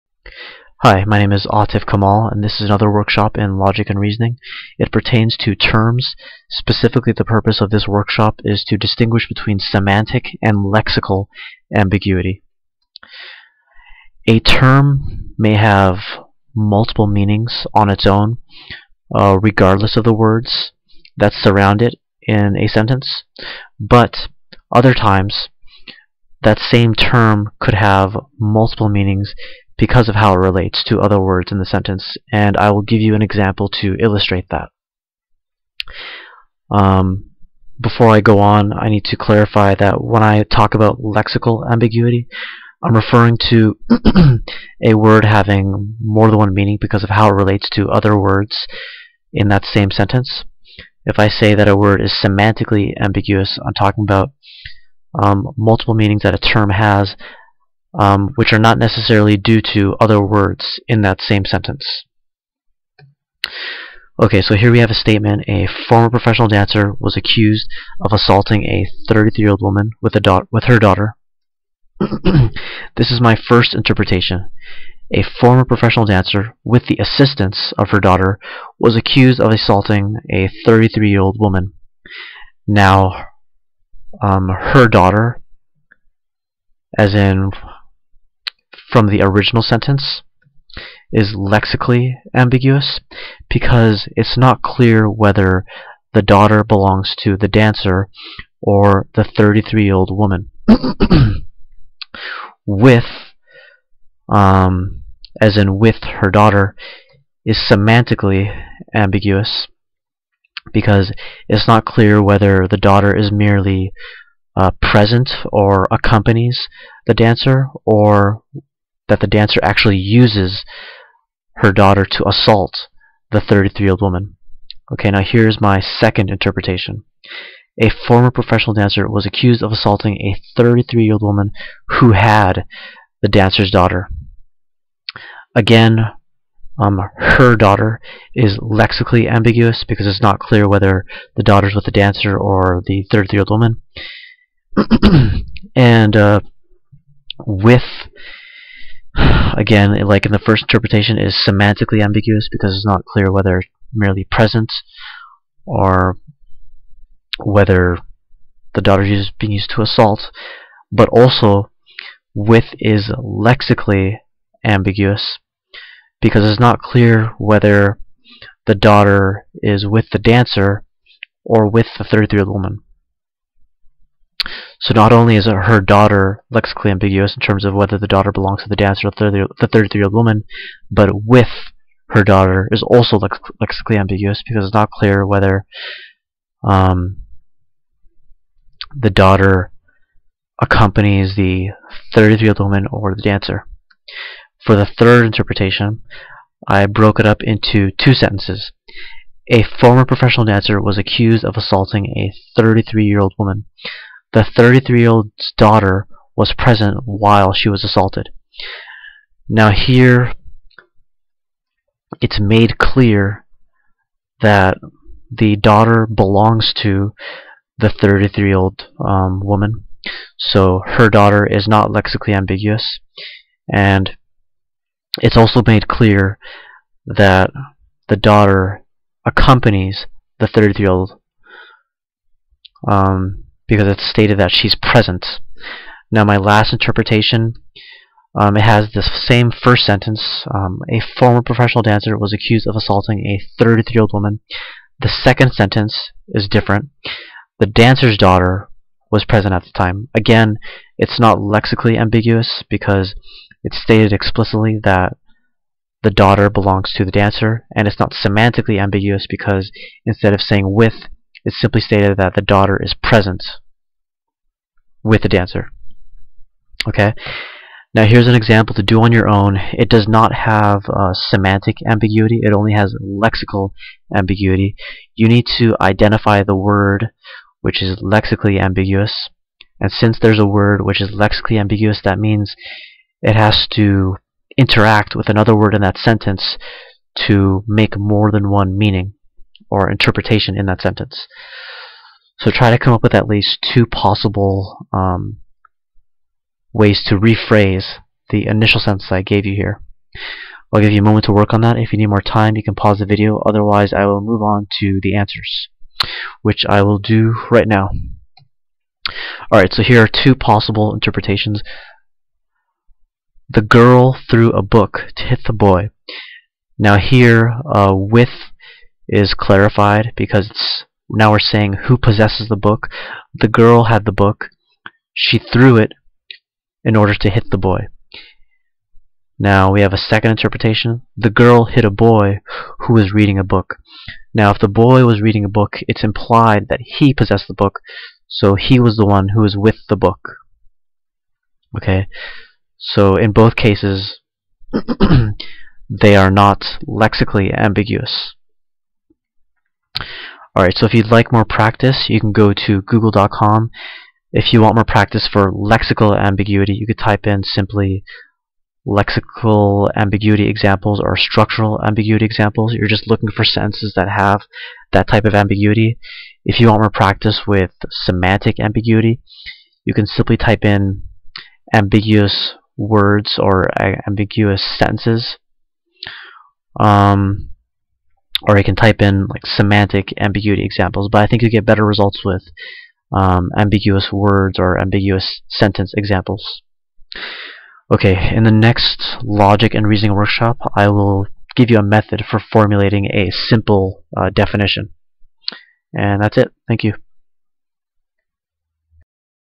Logic & Reasoning Terms Semantic vs Lexical Ambiguity
Autif Kamal talks about words that are ambiguous because of the context of the sentences they occur in (i.e. Lexical) and words that are ambiguous independent of the context they occur in (i.e. Semantic). He exemplifies the numerous interpretations that a lexically ambiguous sentence can lead to.